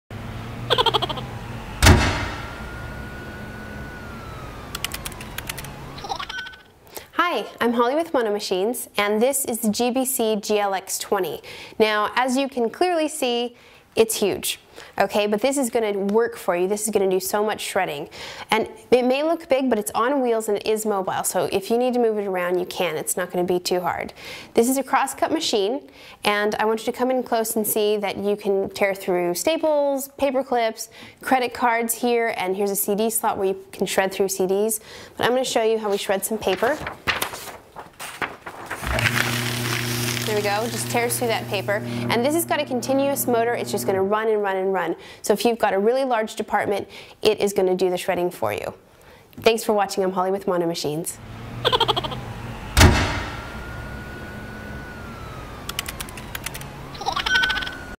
GBC GLX20 Cross-Cut Shredder
--Quick Overview--
-GBC Shredmaster Office Shredder
-Auto Start/Stop and Reverse
-Shreds paper clips, staples, credit cards and CDs
-Environmental feature Auto shut-off saves energy

--Additional Information--
Manufactured In  China
Manufacturer  GBC
Manuf Part #  1770040
Model  GLX20
Weight  93.0000
Dimensions  35.67" x 21.25" x 20.75"
Expected Ship Date  7-10 Days
Shipping Weight  93
Warranty  Lifetime on cutters/2 years on machines including on-site service
Cut Type  cross-cut
Security Level  3
Shred Width  1/6
Shred Length  1 1/2
Sheet Capacity (maximum)  20
Bin Capacity (gal)  30
Throat Size  10.2
Can Shred:  CDs, Credit Cards, Paper, Paper Clips, Staples
Horse Power  0
Continuous Motor  Yes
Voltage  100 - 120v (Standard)